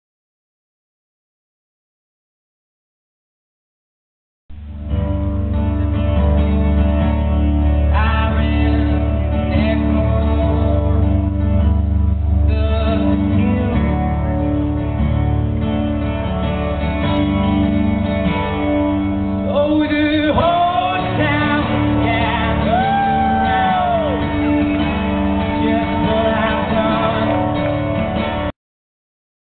Sawyer Fredericks at Outside The Box
A short clip of Sawyer Fredericks performing on July 15, 2015 at Outside The Box in Boston, Massachusetts.